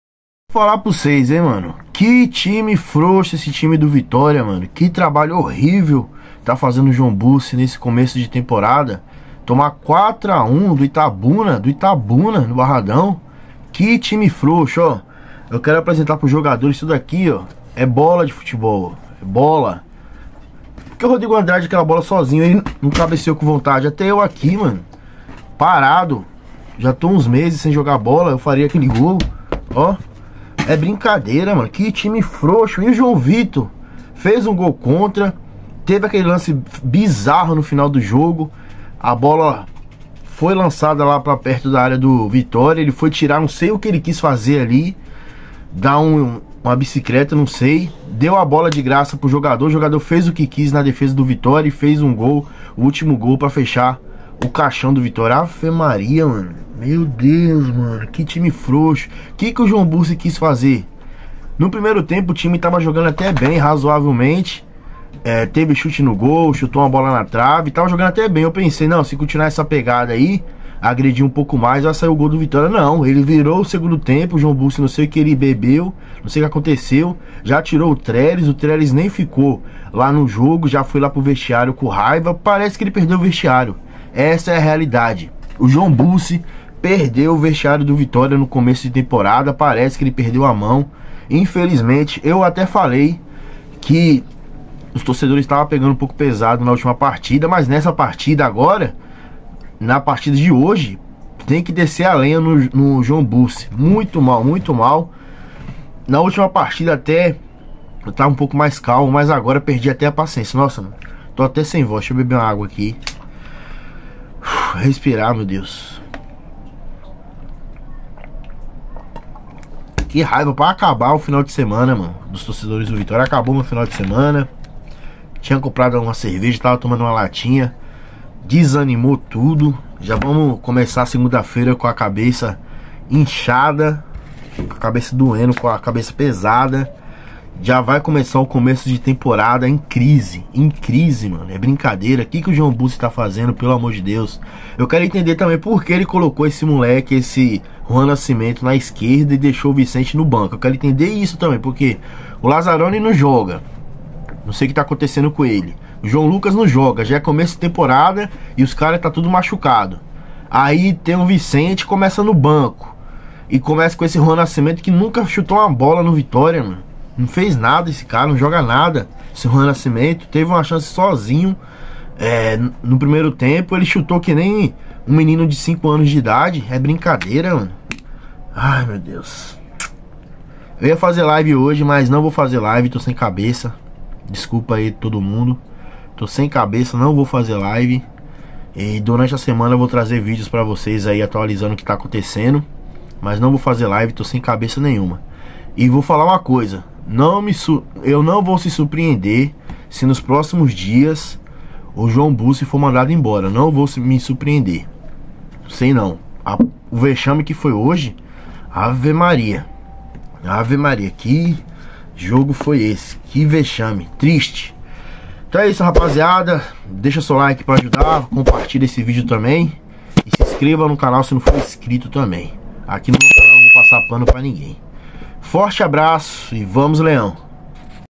VEXAME HISTÓRICO - VITÓRIA 1 X 4 ITABUNA - JOÃO BURSE VAI SER DEMITIDO?
👉🏽Se Inscrevam
👉🏽Compartilhar

SEJAM MEMBROS DO CANAL

Benefícios exclusivos:

👉🏽Vídeos exclusivos
👉🏽Sorteios